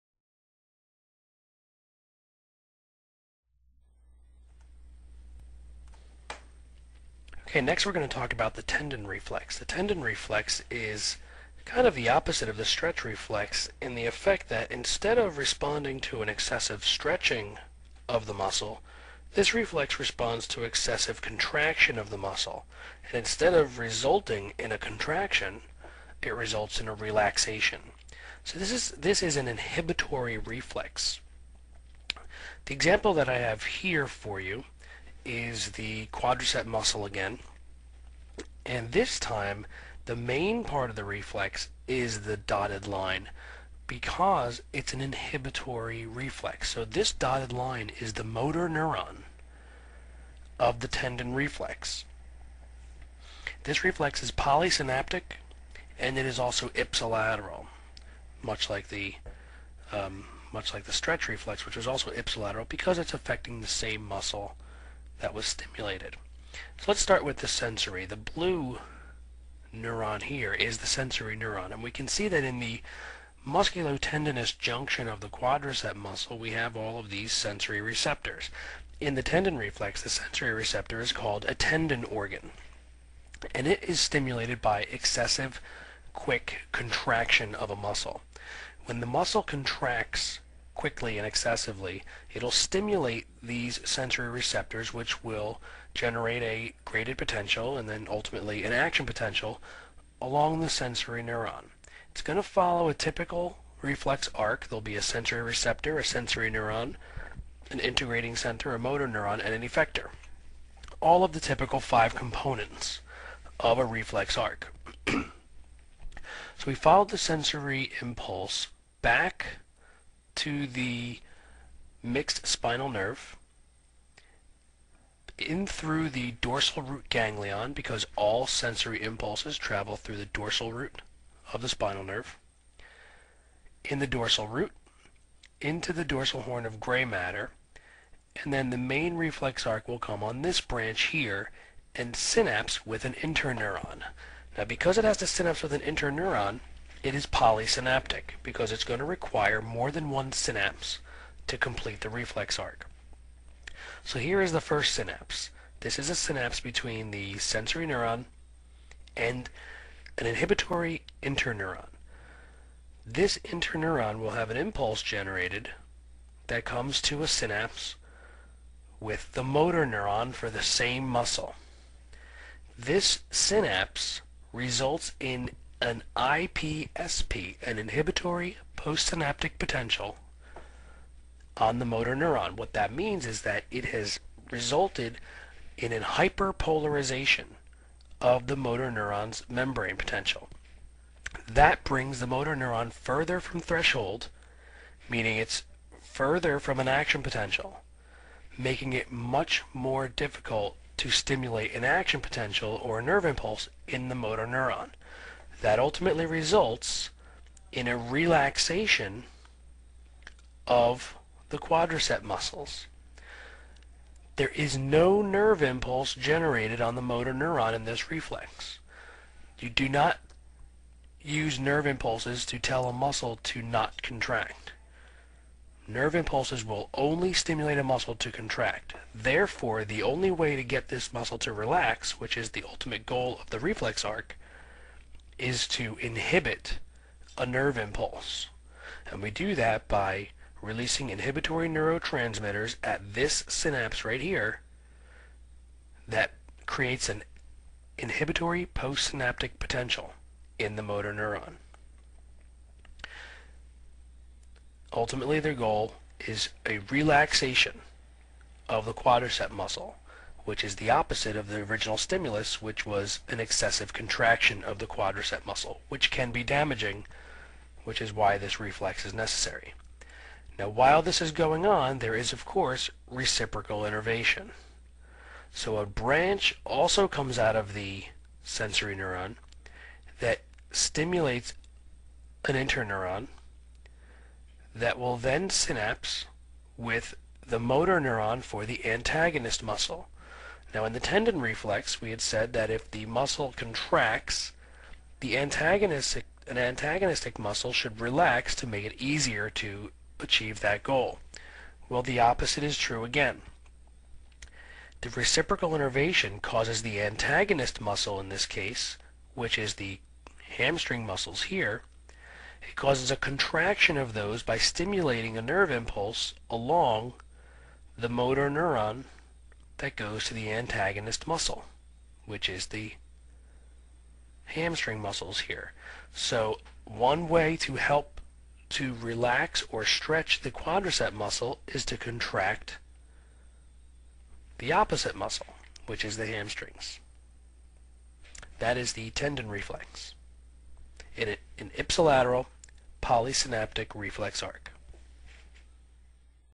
The Tendon Reflex
This is a step by step description of the tendon reflex.